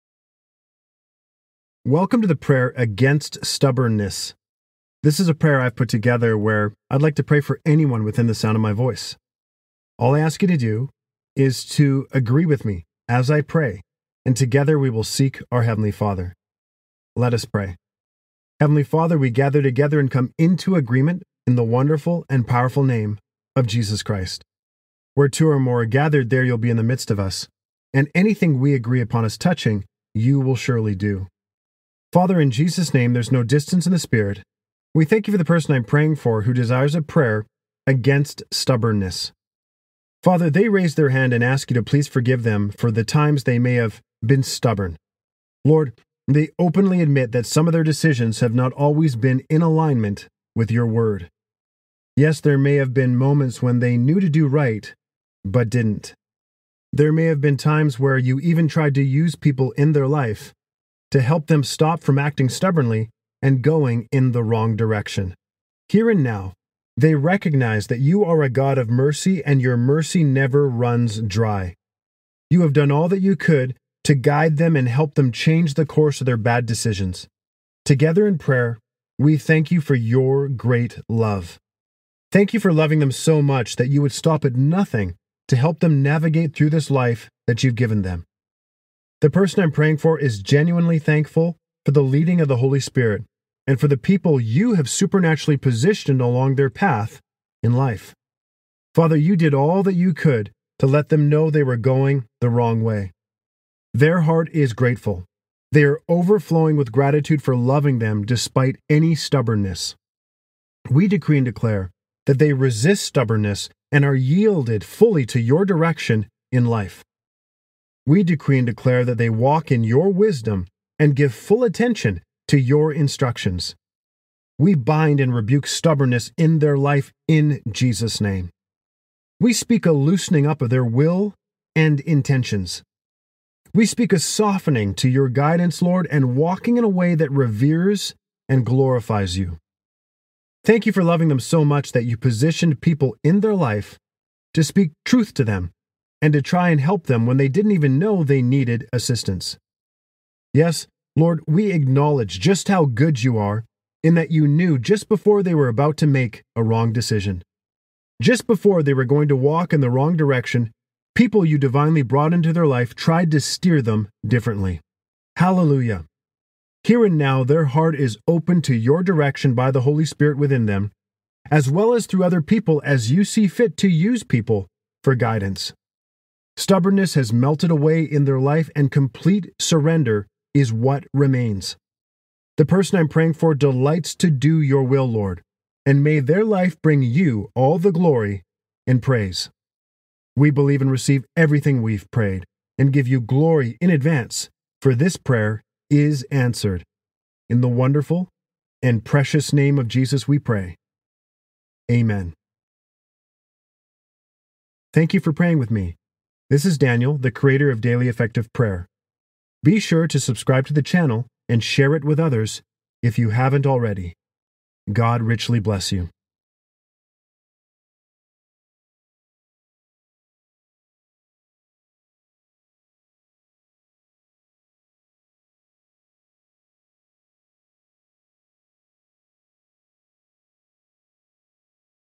Prayer Against Stubbornness | Prayer To Overcome Being Stubborn
Speak it daily or listen to this video over and over again and allow the Word of God concerning this prayer to surrender all to God to reach deep into your spirit.

Powerful Prayer Of Surrender To God
Prayer For Stubborn Son, Husband, and Deliverance From Pride